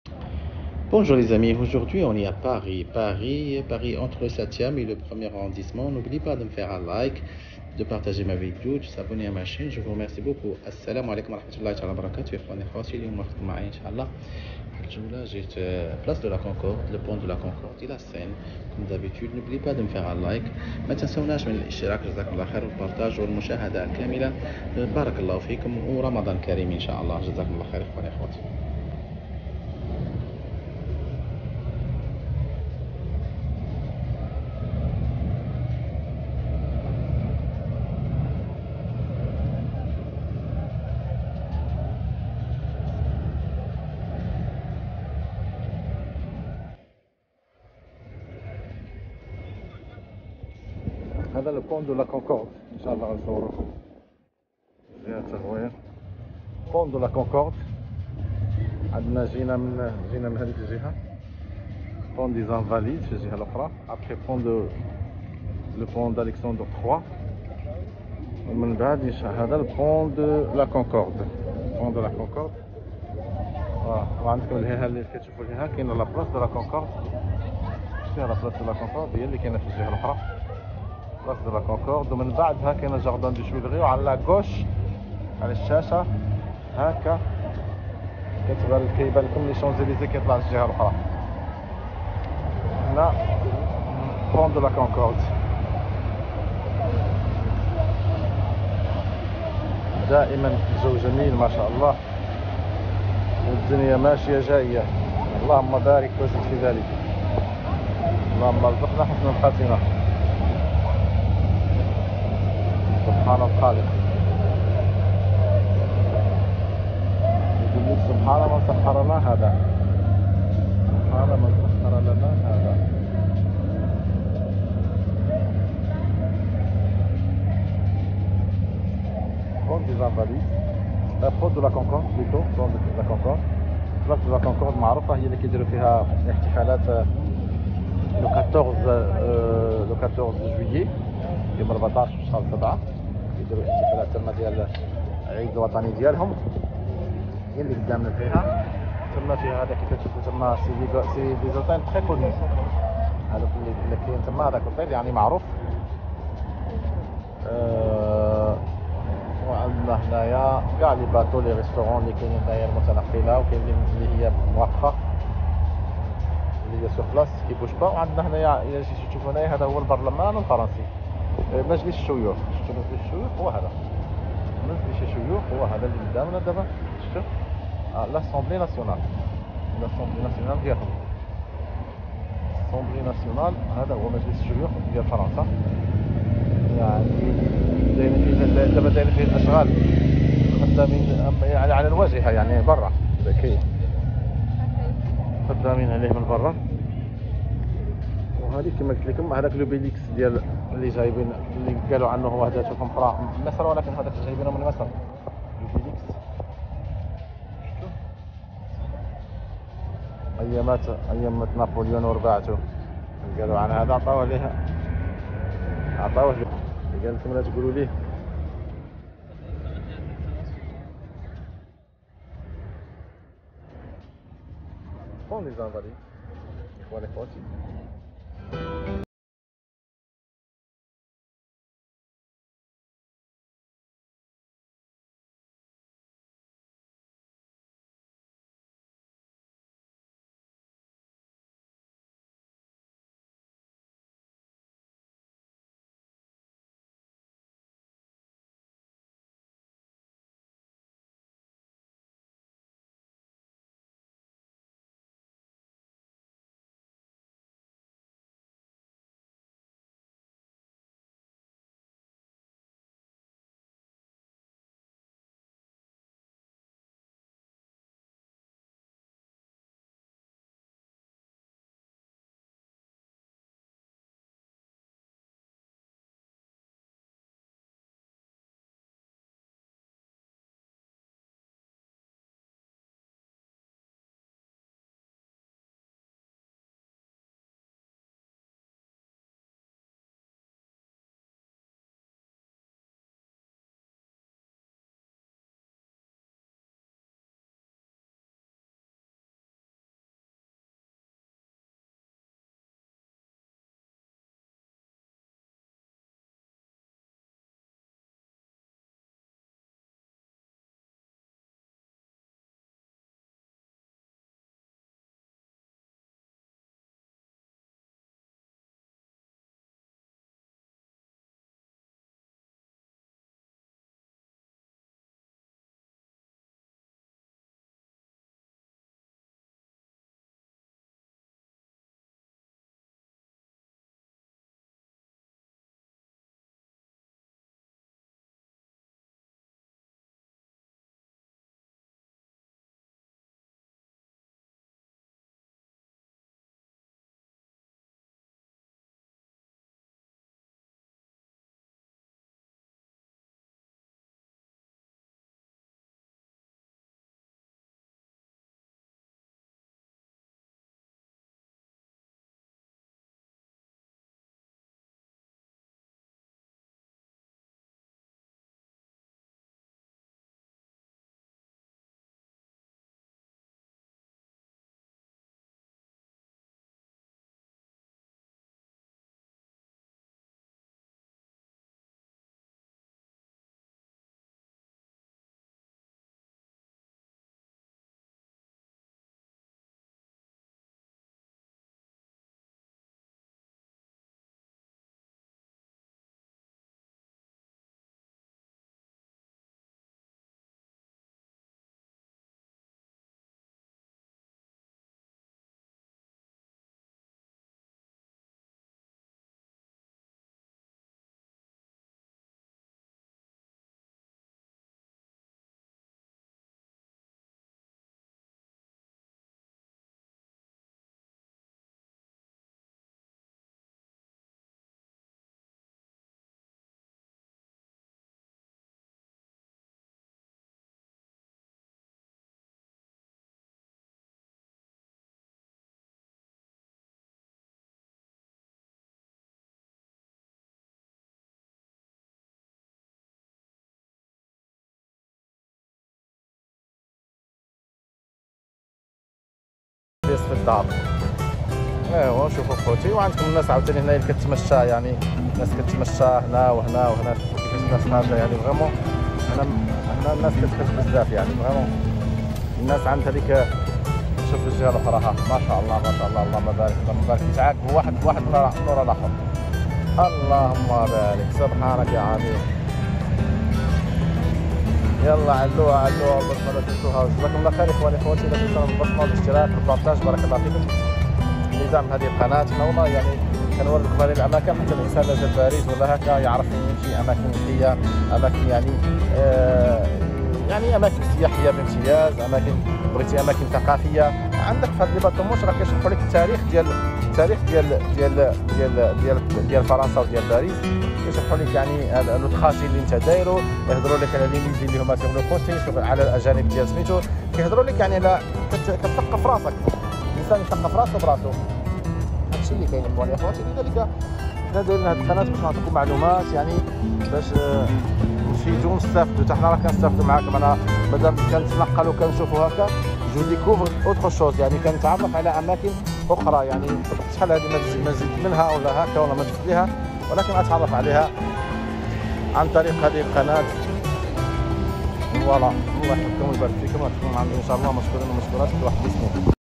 A tour of the seventh arrondissement in Paris, the most beautiful places احلى جولة في شوارع باريس
جولة في الدائرة السابعة في باريس اجمل الاماكن

A tour of the seventh arrondissement in Paris, the most beautiful places